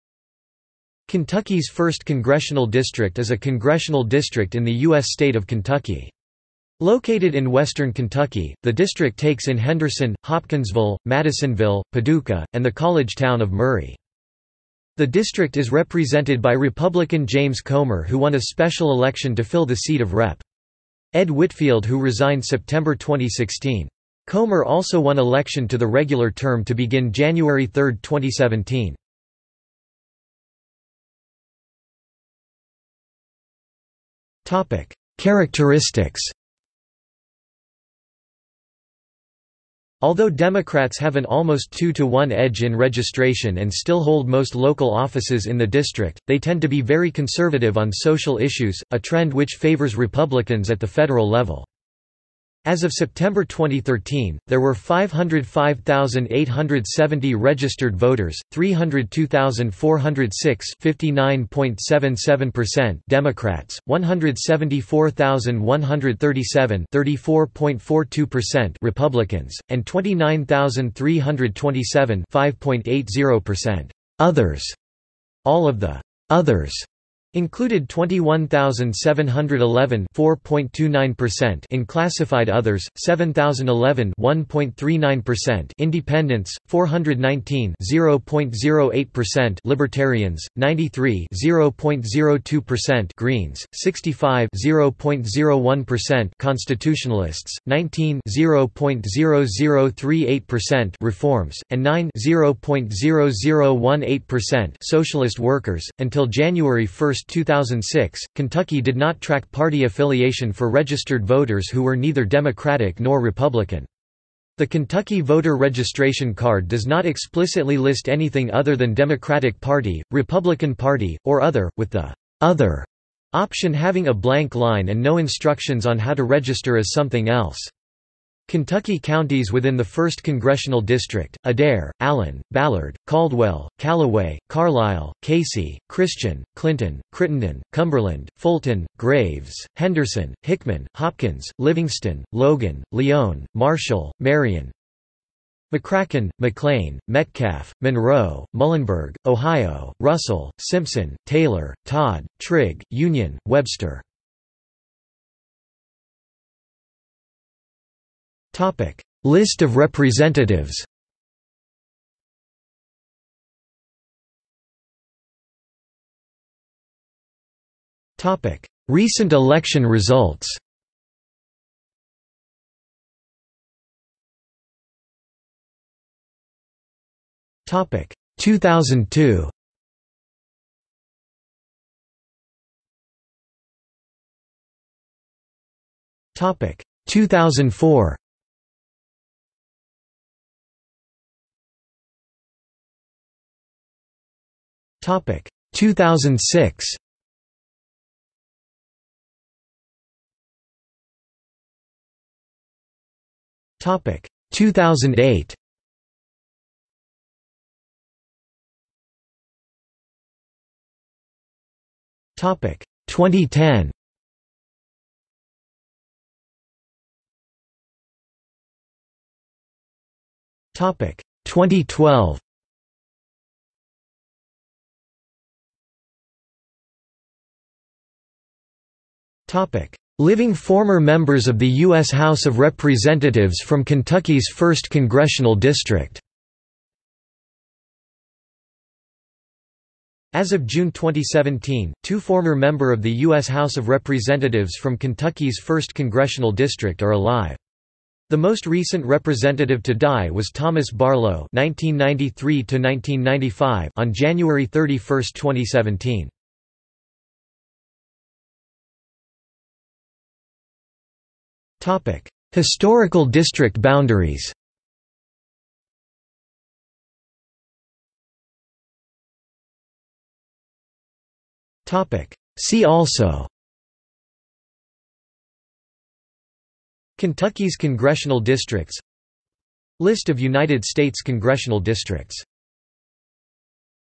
Kentucky's 1st congressional district | Wikipedia audio article
This is an audio version of the Wikipedia Article:
Kentucky's 1st congressional district

00:00:30 1 Characteristics
00:02:34 2 List of representatives
00:02:44 3 Recent election results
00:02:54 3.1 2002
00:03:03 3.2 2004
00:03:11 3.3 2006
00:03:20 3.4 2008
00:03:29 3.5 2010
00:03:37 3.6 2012
00:03:45 4 Living former Members of the U.S. House of Representatives from Kentucky's 1st congressional district
00:04:18 5 Historical district boundaries
00:04:28 6 See also

- Socrates

SUMMARY
Kentucky's 1st congressional district is a congressional district in the U.S. state of Kentucky. Located in Western Kentucky, the district takes in Henderson, Hopkinsville, Madisonville, Paducah, and the college town of Murray.
The district is represented by Republican James Comer who won a special election to fill the seat of Rep. Ed Whitfield who resigned September 2016.  Comer also won election to the regular term to begin January 3, 2017.